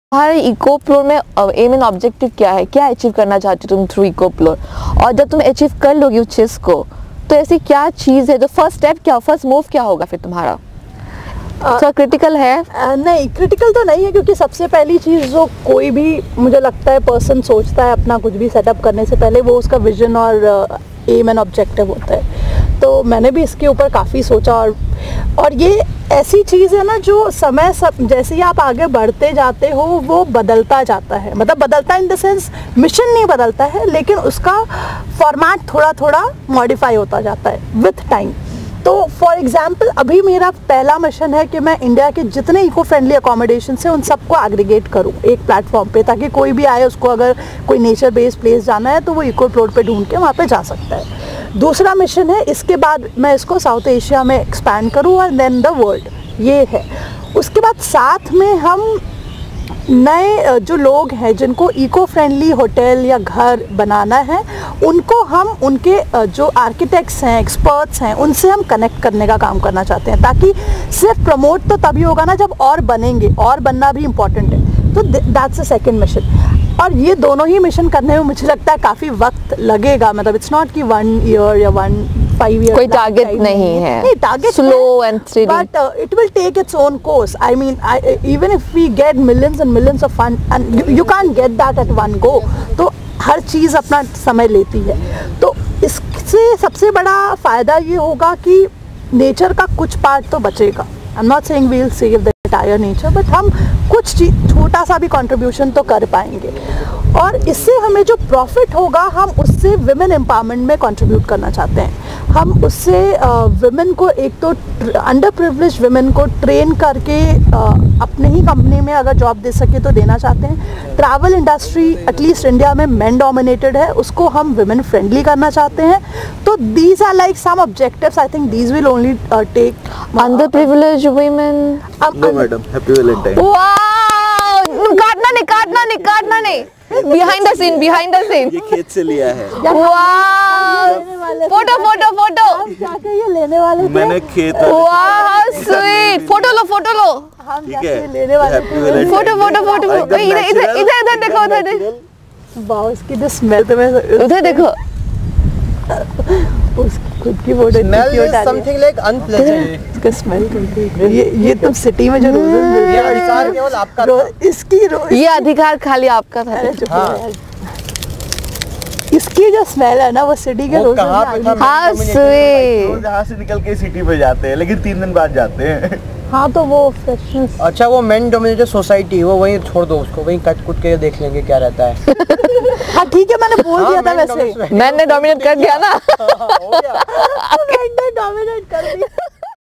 Valentines Day with ECOPLORE 2019 | India's largest Eco Travel Company
At vegetable garden in the outskirts of Delhi.